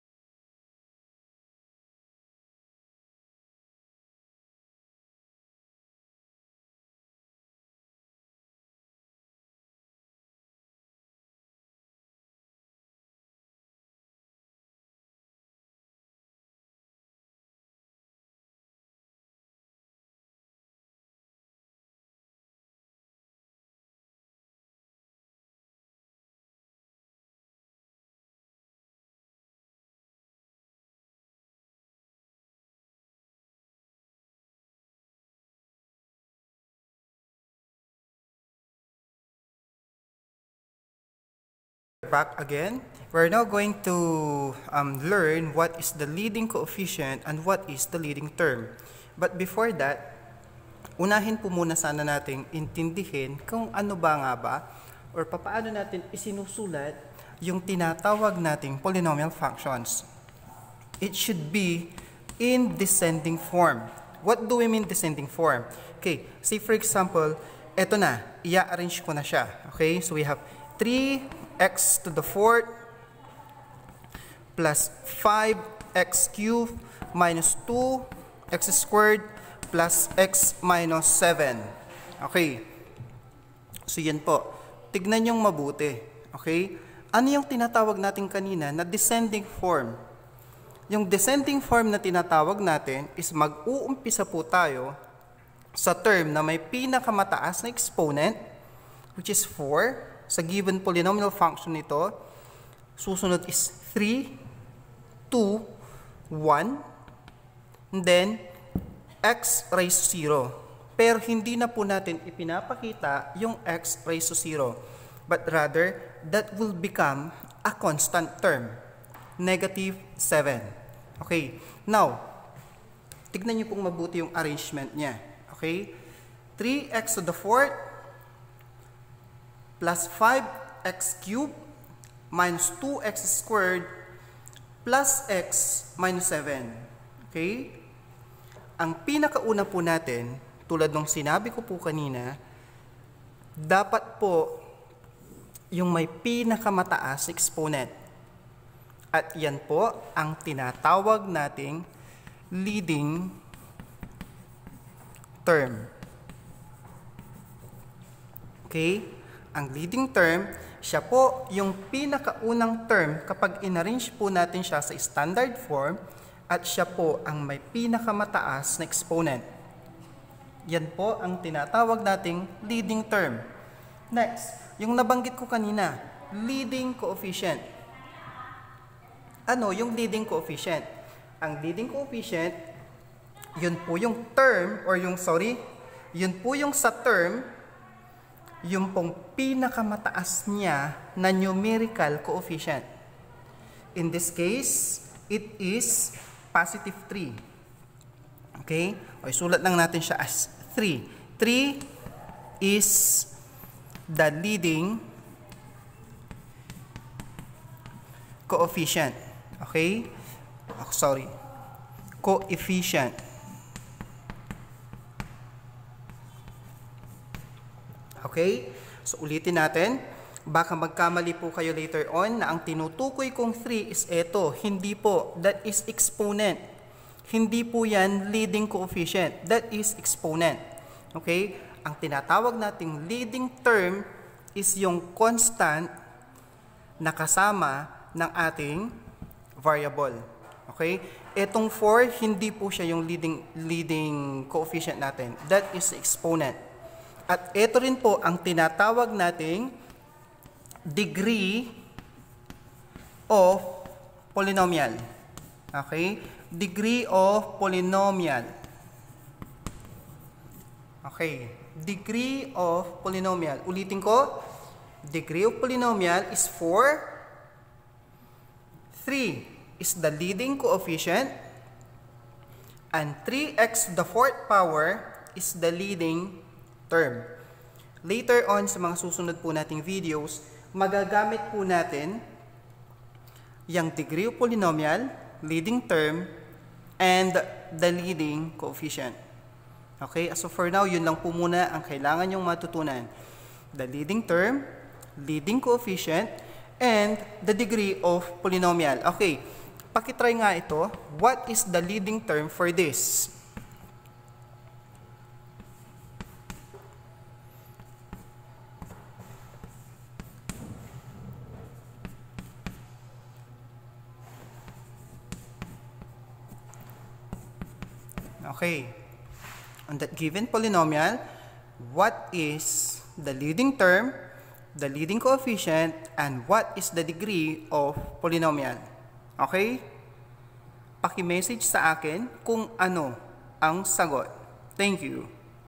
MATH 10 - Kabandaan Leading Term, Leading Coefficient, and Degree of Polynomials | Sir Alos TV
This is a basic discussion about Leading Term, Leading Coefficient and Degree of Polynomials.

Please be guided that this discussion is just a basic.